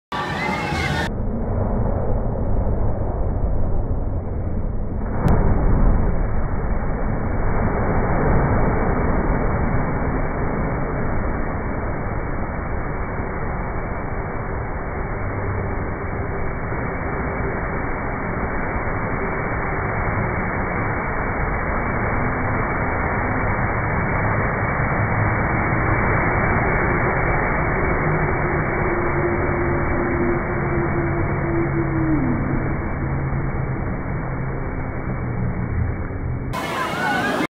Mirabilandia Acqua Splash 2018
L’impatto con l’acqua in ripresa slow motion